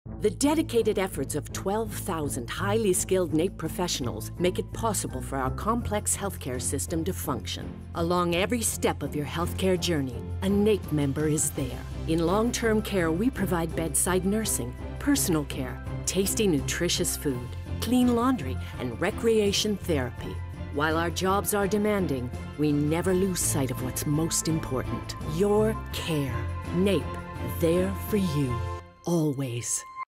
Ad 4. There for You. Always.
The dedicated efforts of 12 thousand highly skilled NAPE professionals make it possible for our province's complex healthcare system to function. In long-term care, NAPE members provide bedside nursing. Personal care. Tasty, nutritious food. Clean laundry. And recreation therapy.